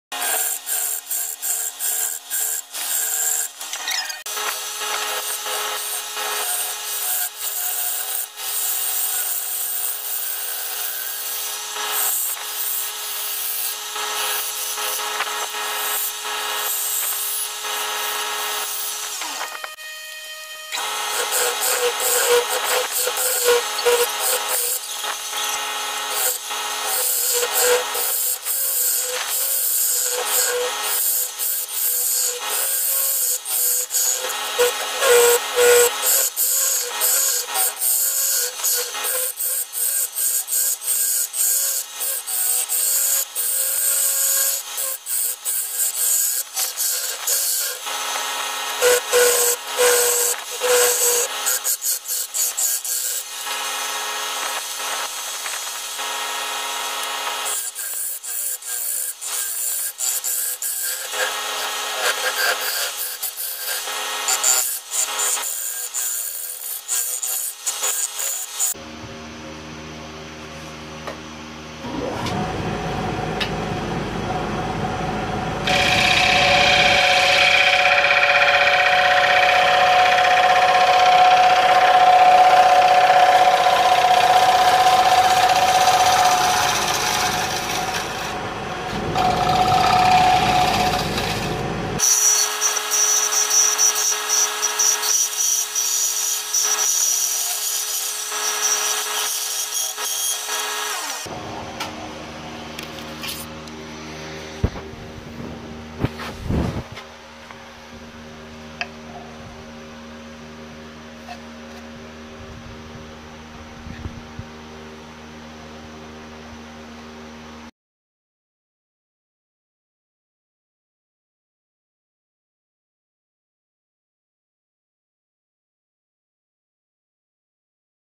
Woodturning a figured crotch bowl
I was gifted this piece of wood and told it was walnut. When i cut it open i started to doubt that it was walnut but its still a nice looking piece